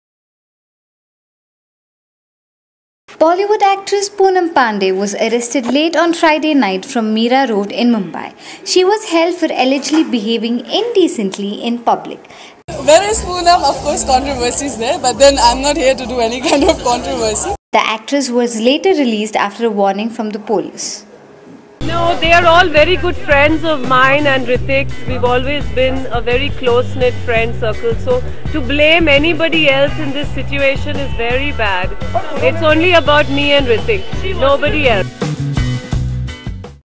Poonam Pandey Arrested For Indecent Behaviour In Public | Silly Monks
Bollywood actress Poonam Pandey was arrested late on Friday night from Mira road in Mumbai. She was held for allegedly behaving indecently in public.

The actress was later released after a warning from the police. More details of the incident are yet to emerge.

Poonam Pandey was last seen in Bollywood film 'Nasha'.